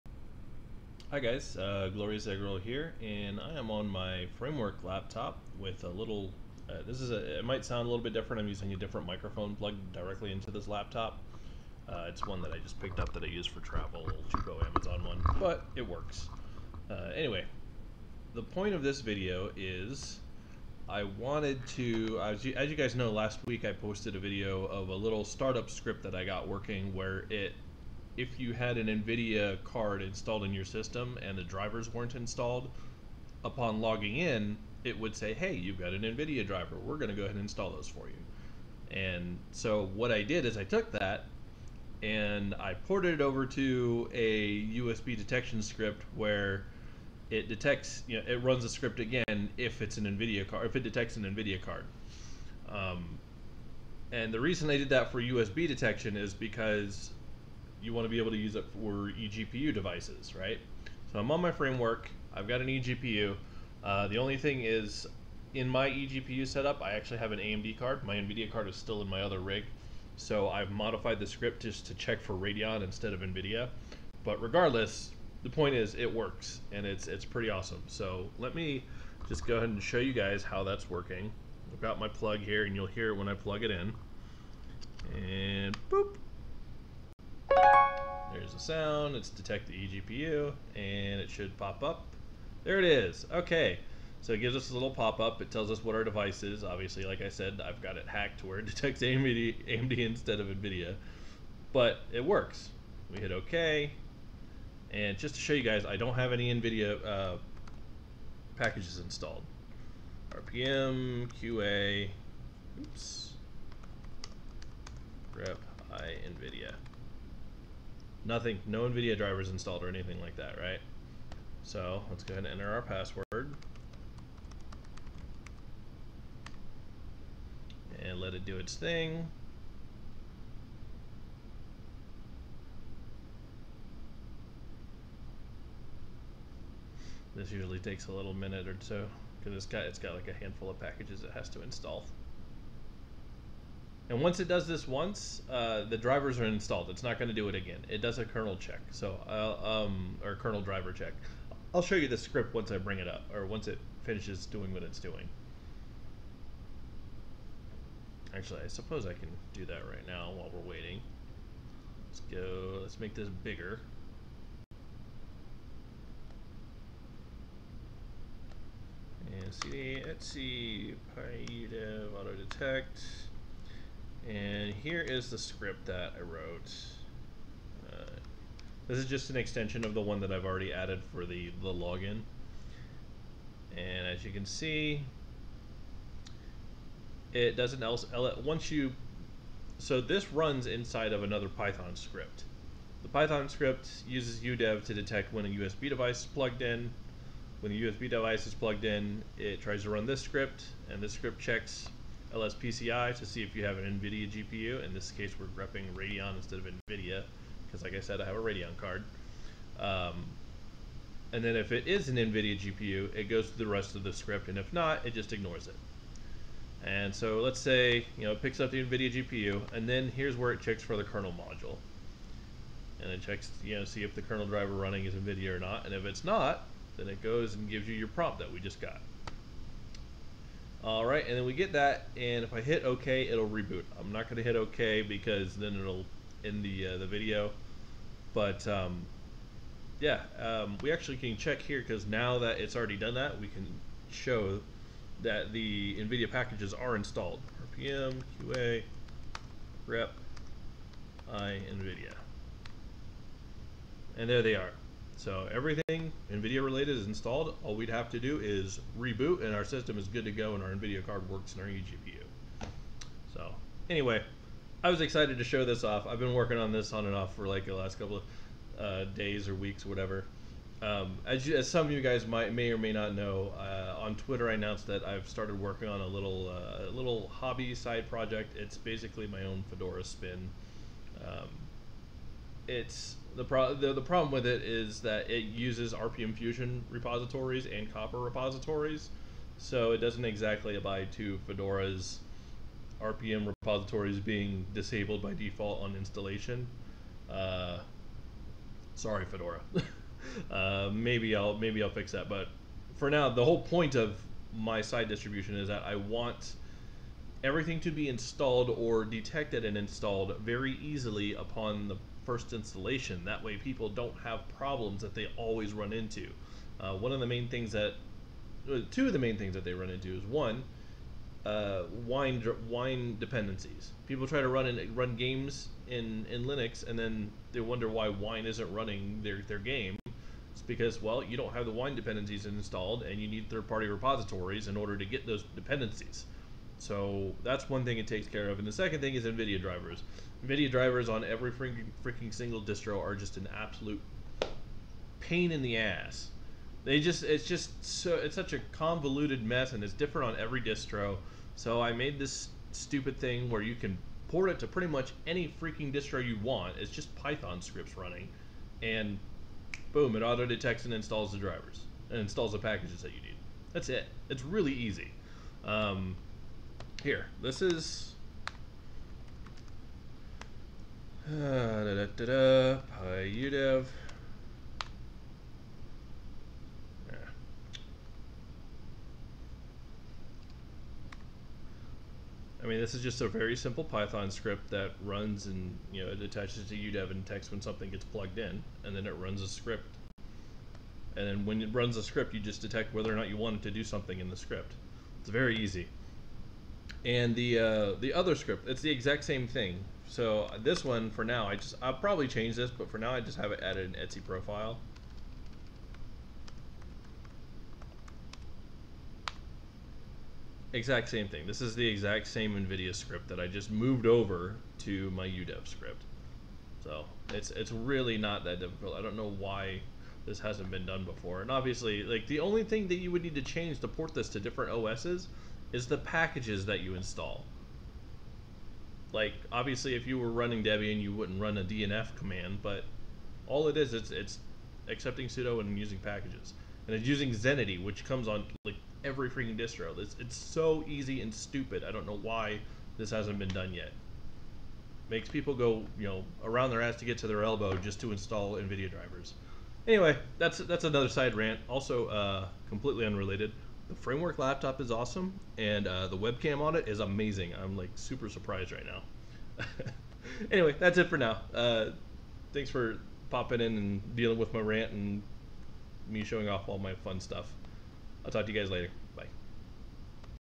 Fedora nvidia auto-detect and driver install script for eGPUs
Last week I showed off an nvidia auto-driver installer I made that comes up on first login of a new Fedora installation. In this video I've ported that over to an auto-detect script for doing the same with eGPUs.